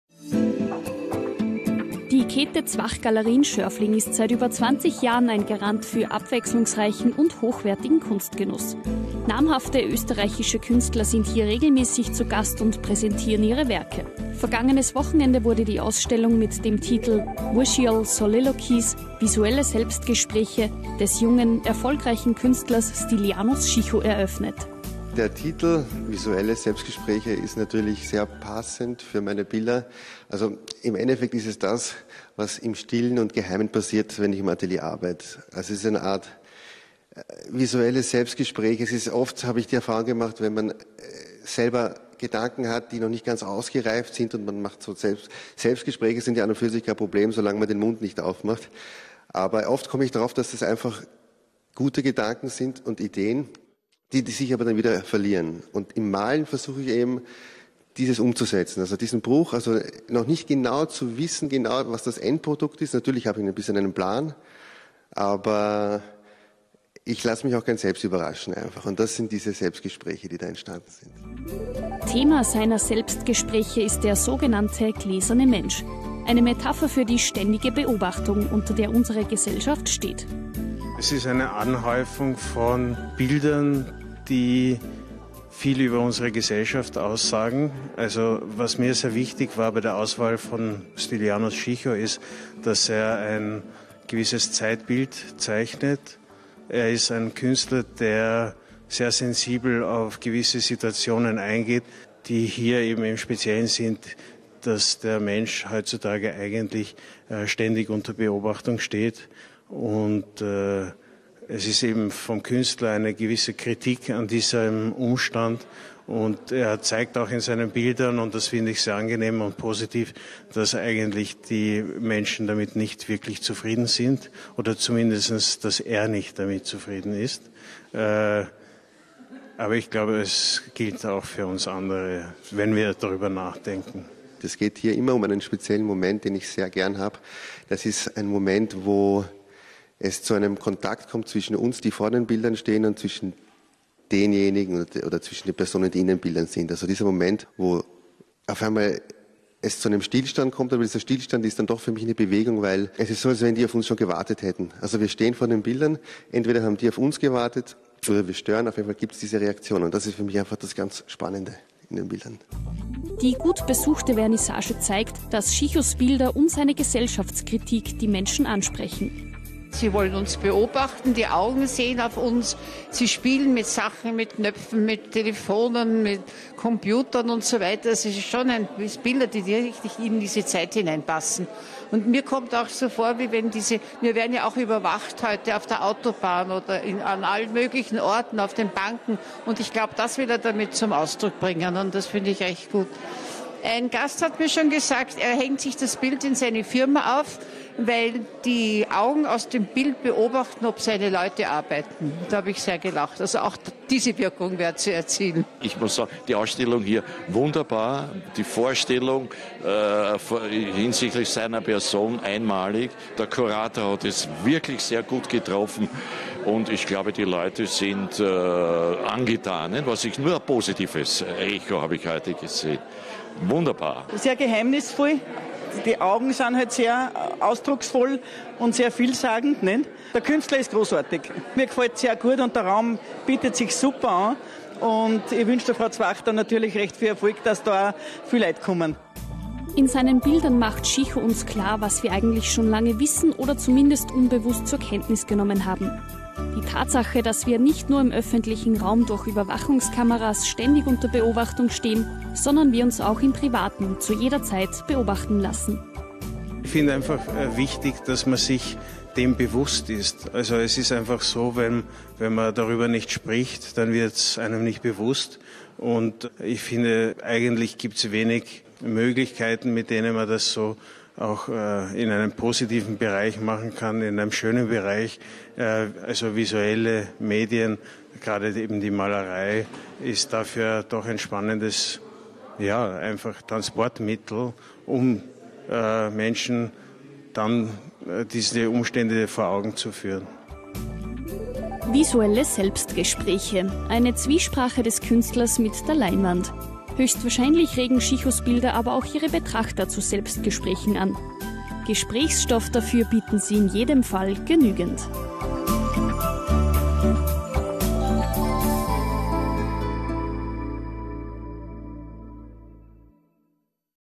stylianos schicho „Visual Soliloquies" Galerie Kaethe Zwach 2013
Vernissage „Visual Soliloquies -- Visuelle Selbstgespräche" Galerie Kaethe Zwach 2013
(BTV - Stylianos Schicho in der Kaethe Zwach Galerie)

Bericht: Christina Gneiß
Kamera: Alwis Wiener
Schnitt: Roswitha Hummelbrunner

Stylianos Schicho "Visual Soliloquies"
Galerie Kaethe Zwach, Schörfling am Attersee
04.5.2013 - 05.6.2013
Kurator: Lucas Cuturi